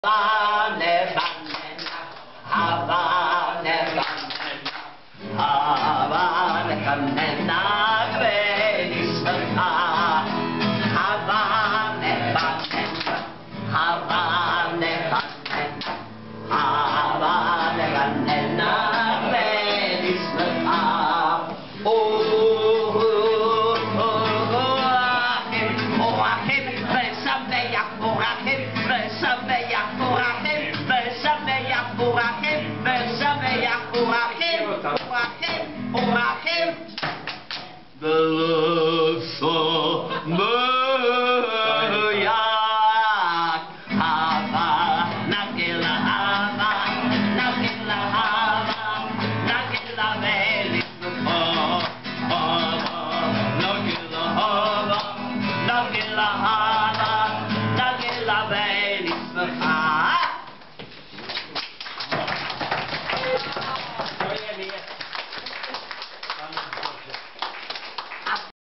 Věra Nerušilová na přivítání knihy Mír v Izraeli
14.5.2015 v podniku Kuře v hodinkách zpívá židovské písně.